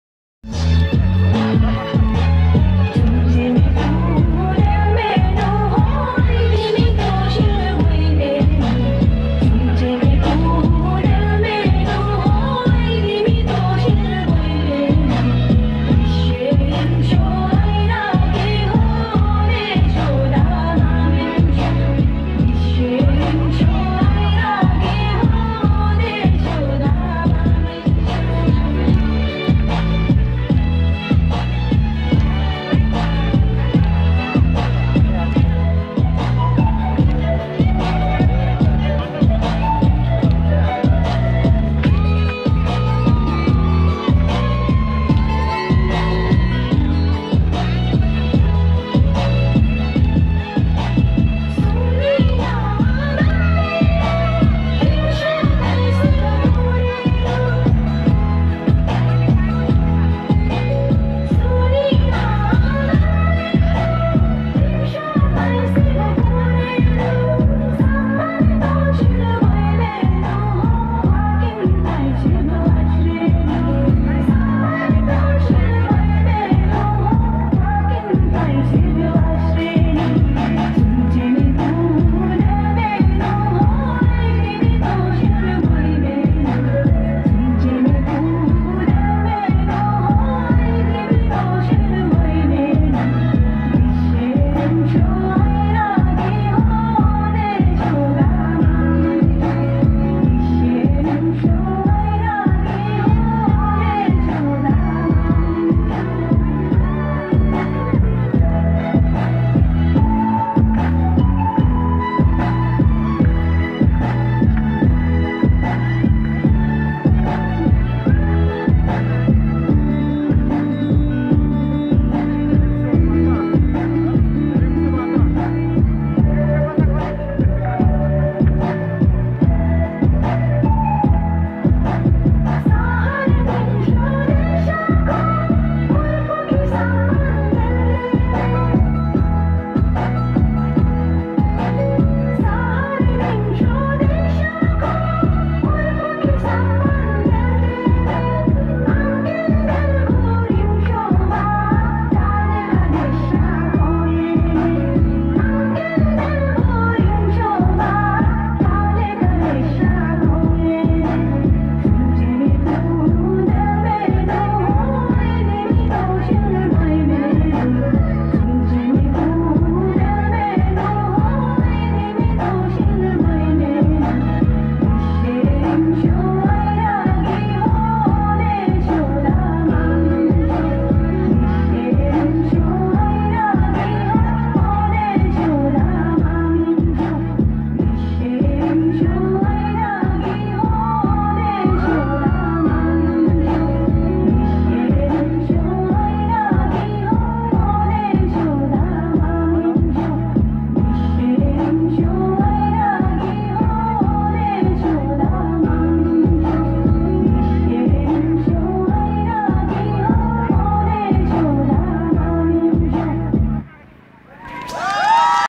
Toshil fu by THE UMAKUNDA DEVILS CREW in umakunda 2 bamti
This dance was shown in umakunda mahautsaab 2074 in umakunda 2 ramechhap